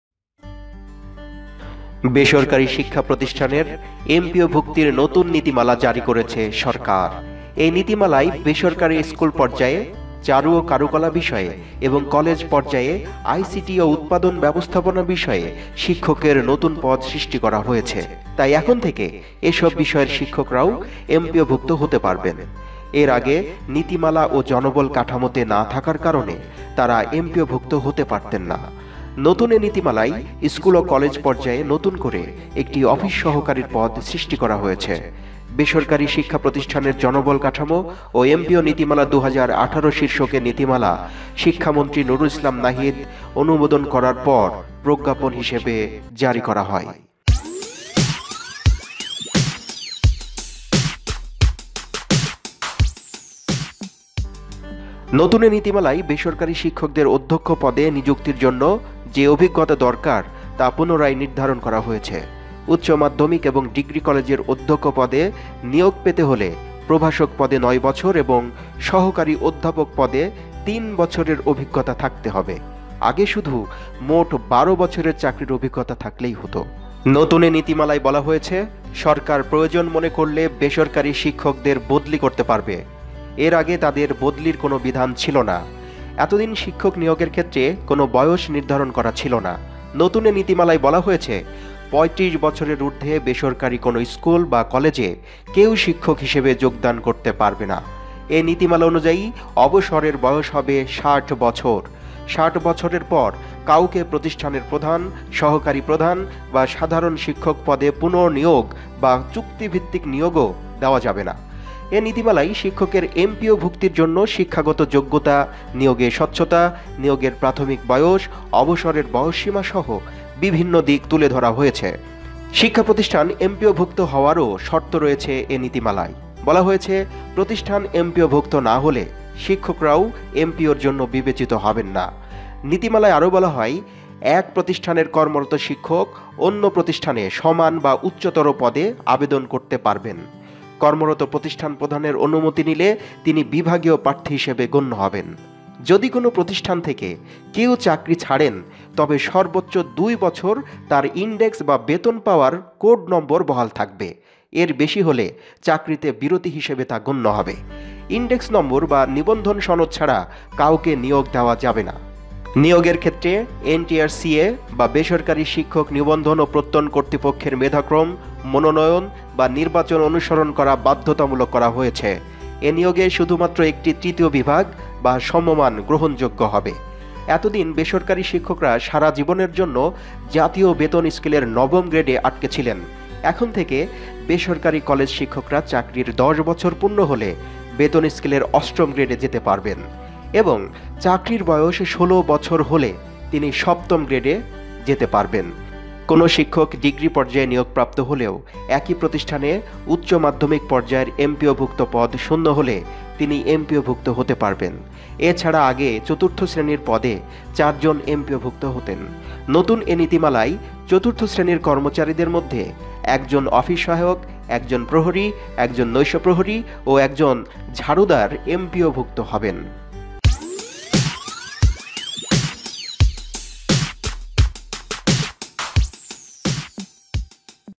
New Rules for Non-government Teachers 2018 | NTRCA | Bangladesh.
New rules for non government teachers. Education minister has declared new law  for the MPO teachers in Bangladesh in June 2018.